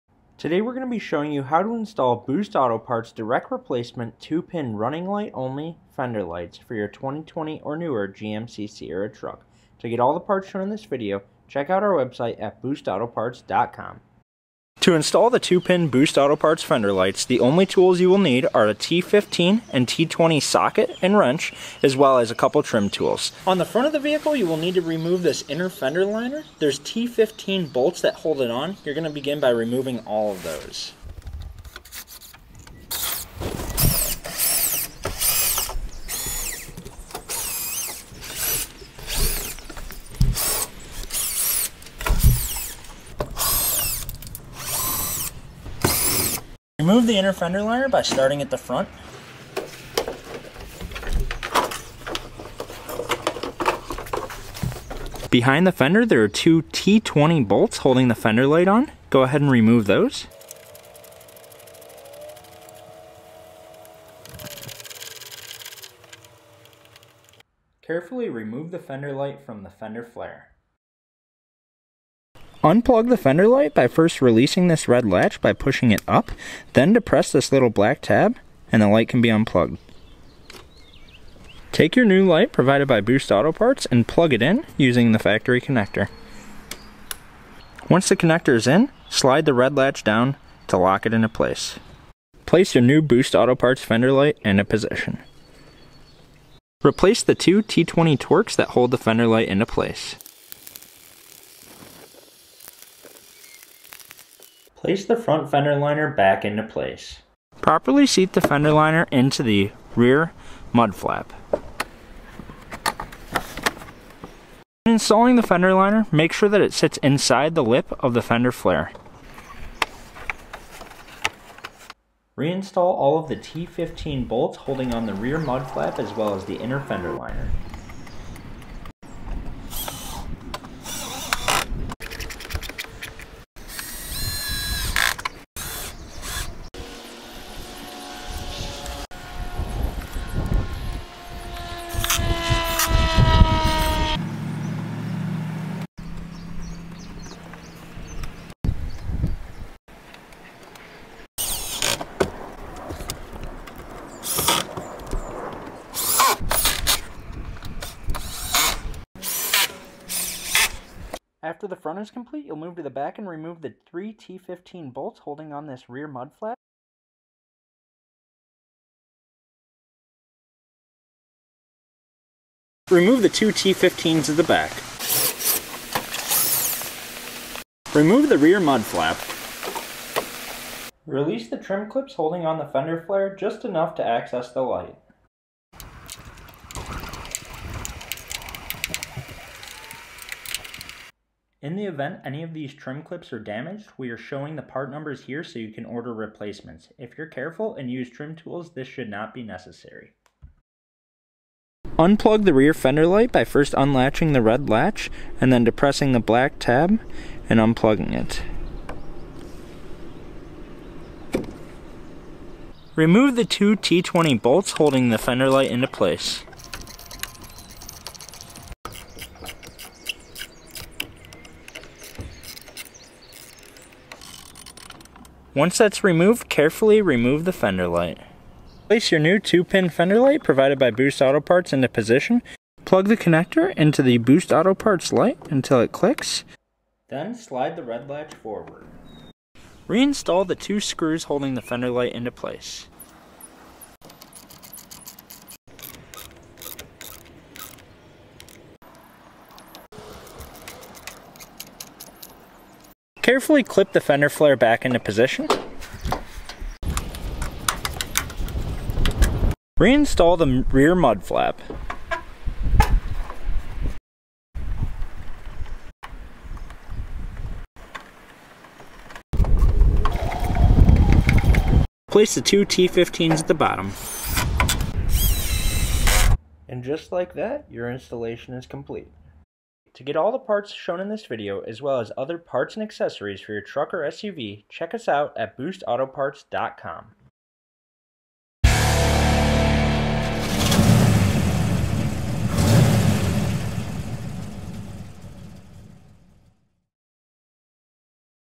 GMC Sierra 2500 / 3500 Fender Light Upgrade - Smoked or Frosted Running Light - (2020+ Sierra)
This video will show you how to replace the fender light on your 2020+ GMC Sierra 2500 or 3500 including Denali. This is applicable for 2500HD and 3500HD including Duramax and gas models. The lights are offered with a smoked or frosted lens.

These are drop in replacement lights for the 2020+ GMC 2500/3500 fender lights. Offered as a direct replacement running light or an upgrade to a running light and sequential turn signal. Completely plug and play; no cutting or splicing whatsoever. Available with a smoked lens or frosted lens, and the included wiring harness makes installation easy.

These replace the factory lights which are part numbers 84185031.

Fitment: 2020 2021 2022 GMC Sierra 2500 or 3500 (including Duramax)